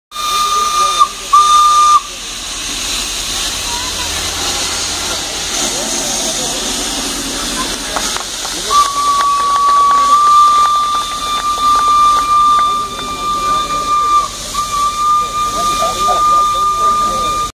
Heritage run of Fairy Queen Steam Locomotive in Railway Loco Workshop, Chennai
Heritage special run of Fairy Queen Steam Locomotive in Loco Workshop, Southern Railway, Indian Railways, Chennai on 26th January, 2016 (Republic Day)